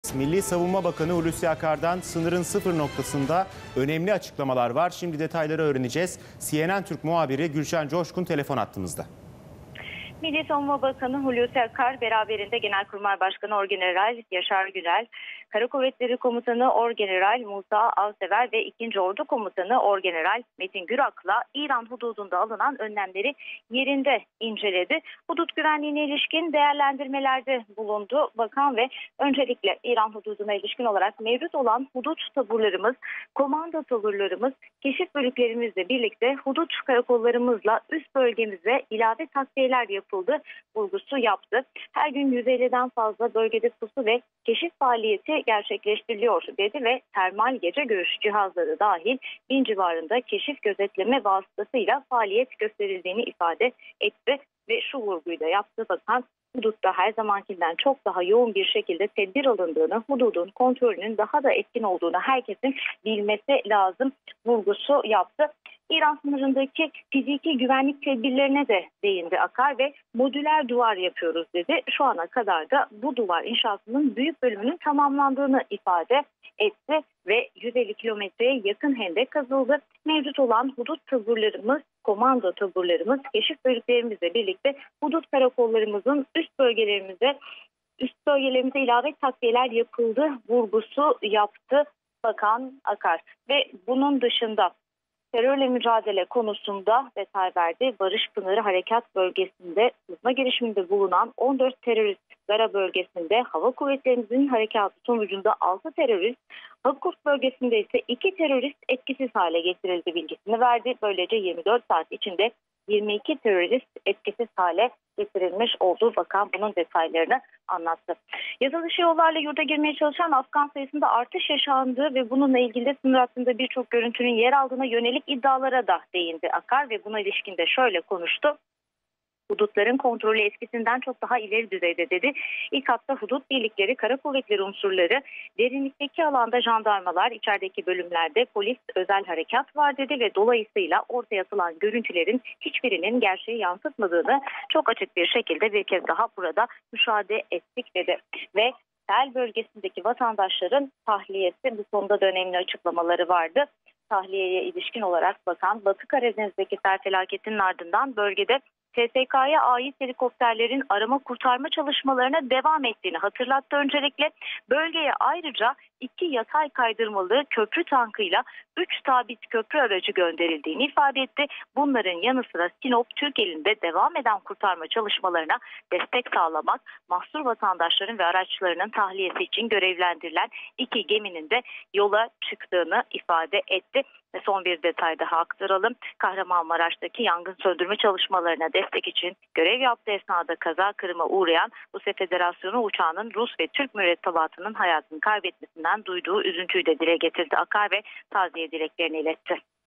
Bakan Akar duyurdu! Son 24 saat içinde 22 terörist etkisiz hale getirildi
Milli Savunma Bakanı Hulusi Akar, yasa dışı yollarla yurda girmeye çalışan Afgan sayısında artış yaşandığı iddialarıyla ilgili "Hudutların kontrolü eskisinden çok daha ileri düzeyde. İlk hatta hudut birlikleri, Kara Kuvvetleri unsurları, derinlikteki alanda jandarmalar, içerideki bölümlerde polis özel harekat var. Dolayısıyla ortaya atılan görüntülerin hiçbirinin gerçeği yansıtmadığını çok açık bir şekilde bir kez daha burada müşahede ettik" dedi. Akar, son 24 saat içinde 22 teröristin etkisiz hale getirildiğini de bildirdi.

Bakan Akar beraberinde Genelkurmay Başkanı Orgeneral Yaşar Güler, Kara Kuvvetleri Komutanı Orgeneral Musa Avsever ve 2'nci Ordu Komutanı Orgeneral Metin Gürak ile İran hududunda alınan önlemleri yerinde inceledi. Havadan ve karadan yaptığı incelemelerin ardından Akar ve Komutanlar, Van Valisi Mehmet Emin Bilmez ve hudut birliklerinin komutanlarıyla 6'ncı Hudut Tugay Komutanlığı’nda bir araya geldi.

'İRAN HUDUDUNDA 61 BİN 943 KİŞİNİN GİRİŞİ ENGELLENDİ'

Hudut güvenliğine ilişkin değerlendirmelerde bulunan Akar, alınan tedbirleri gün boyunca yerinde incelediklerini ifade etti. Hudut birliklerinin yıl başından bu yana yurt genelinde 91 bin 78 kişiyi yasadışı yollarla girmeye çalışırken yakaladığını belirten Akar, "Bunlardan İran hududunda yakalananların sayısı 587'dir. Hudutlarımızdan girmesi engellenenlerin sayısı ise 181 bin 328 oldu. Bunların İran sınırına tekabül edenlerinin sayısı ise 61 bin 943'dür. Bir tarafta hudutta askerler diğer tarafta mülki makamlar diğer tarafta ise cumhuriyet savcılıklarımız bu konuyu etkili bir şekilde yürütüyorlar" dedi.

'İLAVE TAKVİYELER YAPILDI'

İran sınırındaki fizik güvenlik tedbirlerine de değinen Akar, "Modüler duvar yapıyoruz, şu ana kadar da bu duvar inşasının büyük bölümü tamamlandı. 150 kilometreye yakın hendek kazıldı. Mevcut olan hudut taburlarımız, komando taburlarımız, keşif bölüklerimizle birlikte hudut karakollarımızın, üs bölgelerimize ilave takviyeler yapıldı. Bunların yanı sıra jandarmalar ve polis özel harekat birlikleri de takviye edildi. Hudutta her zamankinden çok daha yoğun bir şekilde tedbir alındığını, hududun kontrolünün daha da etkin olduğunu herkesin bilmesi lazım" diye konuştu.

HER GÜN 150'DEN FAZLA KEŞİF FAALİYETİ'

İran hududunun güvenliği için karadan alınan önlemlerin yanı sıra havada da İHA’ların, İnsanlı Keşif Uçaklarının görev yaptığını vurgulayan Akar, "Ayrıca her gün 150’den fazla bölgede pusu ve keşif faaliyeti gerçekleştiriliyor. Termal gece görüş cihazları dahil bin civarında keşif gözetleme vasıtamızla faaliyet gösteriyoruz. Yapılan elektro optik kuleler ve akustik algılayıcılar sayesinde sınır hattındaki hareketliliği tespit etmek çok kolay hale geldi. 'Hudut namustur' anlayışı ile en kıdemsizinden en kıdemlisine kadar bütün arkadaşlarımız yüksek moral ve motivasyonun yanı sıra büyük bir kahramanlık ve fedakarlıkla kendilerine verilen görevleri başarıyla yerine getirdiler, getirmeye devam ediyorlar. Çalışmalarımız artan bir şiddetle devam edecek ve 'hudutlarımız geçilmez' anlayışını pekiştireceğiz" ifadesini kullandı.

'24 SAAT İÇİNDE 22 TERÖRİST ETKİSİZ HALE GETİRİLDİ'

Terörle mücadelenin artan bir şiddet ve tempoda aralıksız devam ettiğini vurgulayan Akar, "Barış Pınarı Harekat bölgesinde sızma girişinde bulunan 14 terörist etkisiz hale getirildi. Ayrıca Hava Kuvvetlerimizin harekatı sonucunda Gara bölgesinde 6 terörist etkisiz hale getirildi. Hakurk bölgesinde ise 2 terörist etkisiz hale getirildi. Böylece 24 saat içinde 22 terörist etkisiz hale getirildi. Mücadelemiz devam edecek. Bizim tek hedefimiz teröristler. En son terörist etkisiz hale getirilinceye kadar da mücadele devam edecek. İnşallah asil milletimizi terör belasından kurtaracağız" diye konuştu.

Kahramanmaraş’taki yangın söndürme çalışmalarına destek için görev yaptığı esnada kaza kırıma uğrayan Rusya Federasyonu uçağının Rus ve Türk mürettebatının hayatını kaybetmesinden duyduğu üzüntüyü dile getiren Akar, taziye dileklerini de iletti.